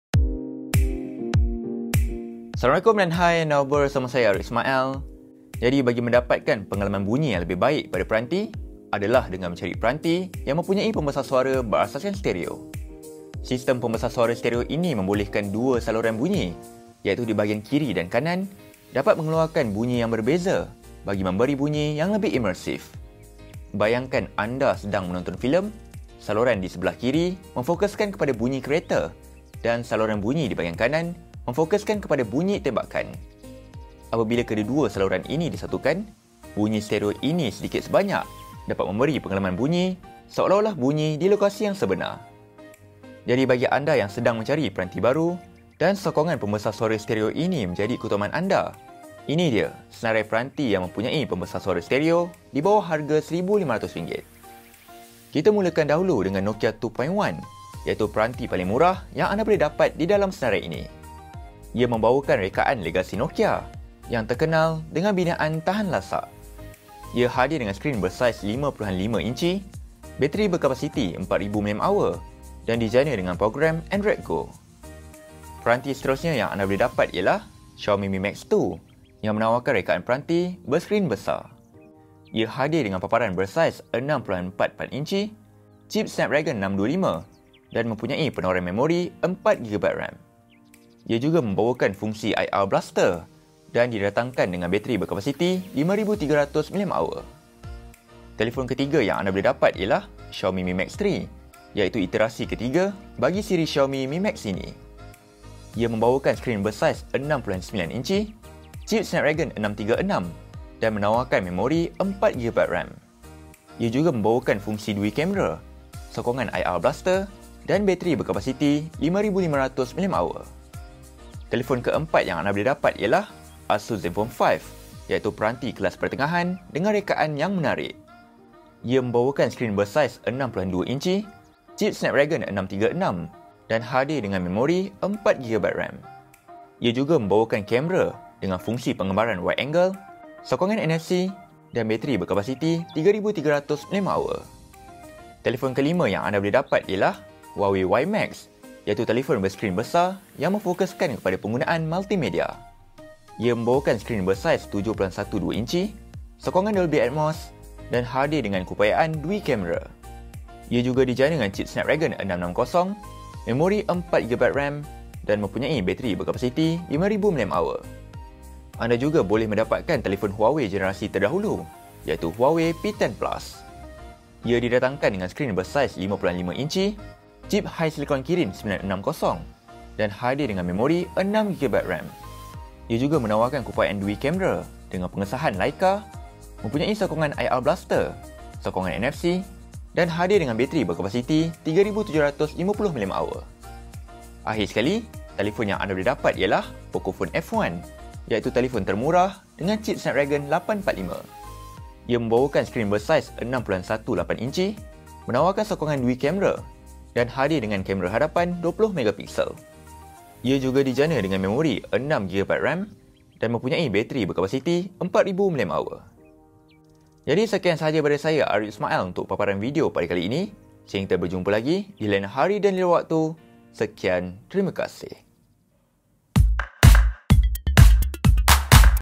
Telefon Dengan Pembesar Suara Stereo Di Bawah RM1500
Bagi mendapatkan pengalaman bunyi lebih baik pada peranti, adalah dengan mencari peranti yang mempunyai pembesar suara berasaskan stereo.

Sistem pembesar suara stereo ini membolehkan dua saluran bunyi, iaitu di bahagian kiri dan kanan, dapat mengeluarkan bunyi berbeza bagi memberi bunyi yang lebih imersif.

Jadi bagi anda sedang mencari peranti baru dan sokongan pembesar suara stereo ini menjadi keutamaan anda, ini dia senarai peranti yang mempunyai pembesar suara stereo di bawah harga RM1500.

Pengulas & Suntingan Video:
• Arib Ismail: